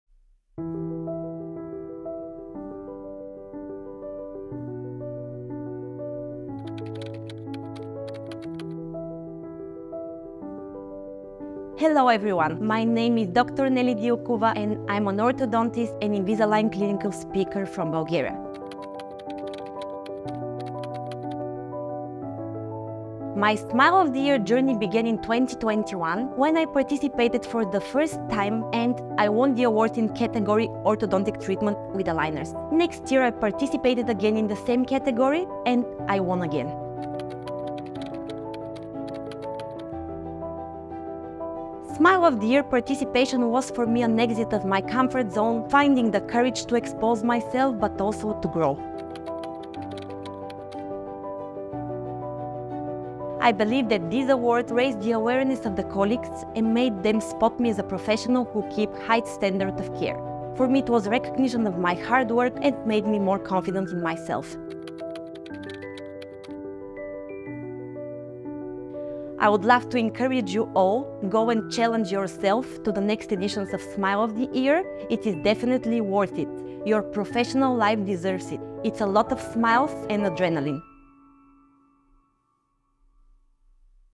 Dr. Nelly Dilkova, shares diamond insights about her "Smile of the Year" journey.
🏆 From Participant to Winner! 🌟The two-times winner, Dr. Nelly Dilkova, shares diamond insights about her "Smile of the Year" journey.

The winners this year will be announced at the Awards Ceremony, which will be held on May 16, 2024, at 8:00 pm.